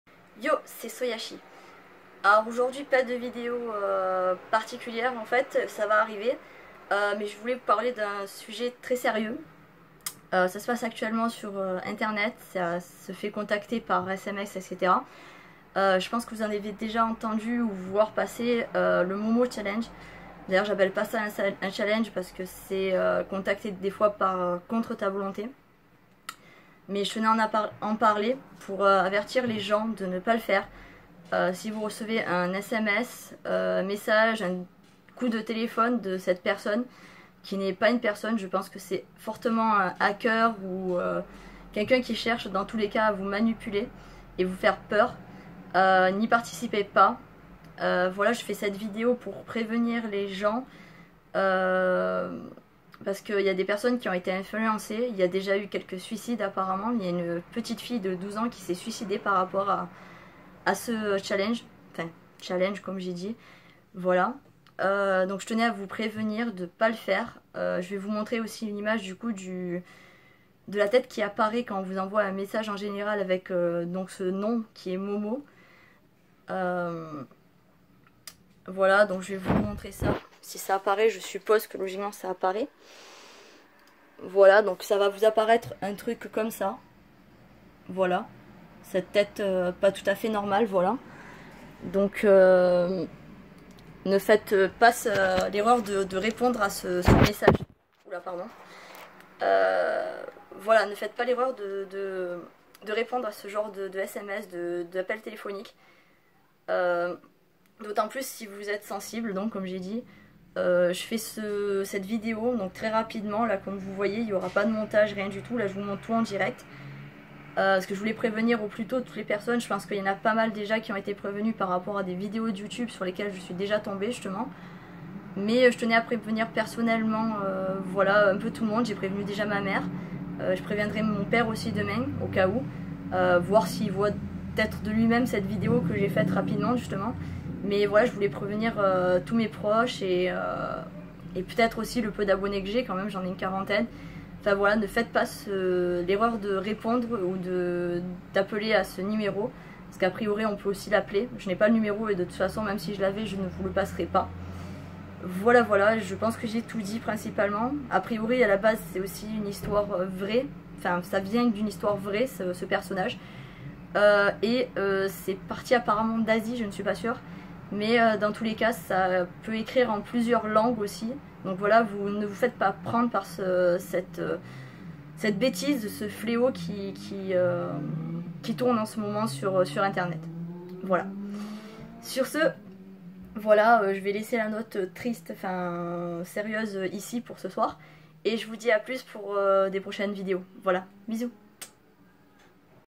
WARNING!!! MOMO CHALLENGE!!!
Je sais que ce ne sera sûrement qu'une goutte d'eau dans l'océan mais, si par cette vidéo, j'ai pu prévenir au moins quelques personnes du danger... Ce sera déjà bien.

Edit: Petite précision que je viens d'avoir, ce "jeu" se passe principalement sur l'application "Whatsapp".